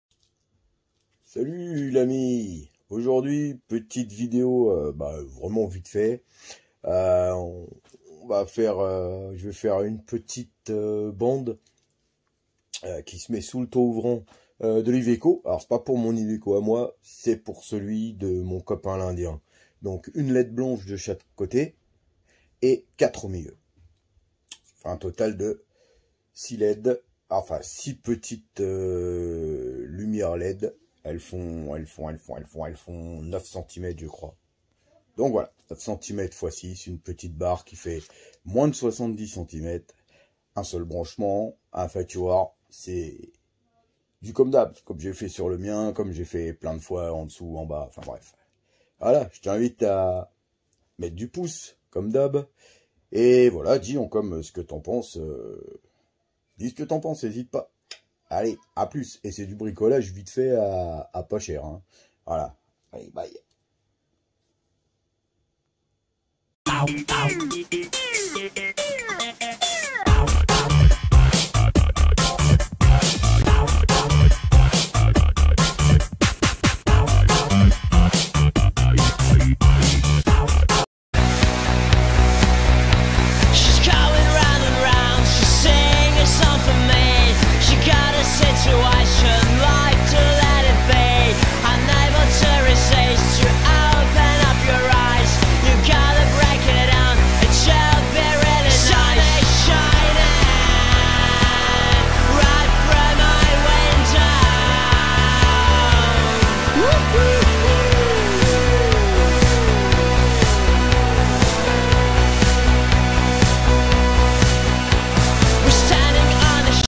SWay , lumière sous toit ouvrant
Fabrication d'une lumière pour mon copain l'indien, barre plastique de 67 cm de long ,2cm de large,et 1 cm de haut,,,, 4 lumières bleu et deux blanche,,2 bonnes heure de taf, plus mise en peinture et séchage ,total , tout l aprèm,,coût,,,ba pas grand choses, lumière à 10 euro les dix chez wish , barre se plastique récup donc 00 euro, peinture à la bombe vite fait. elle se fixe au double face sous le tout ouvrant,donc visible la nuit si allumée, sinon invisible joucomme nuit si éteinte,, du grand Mendes ,LOL.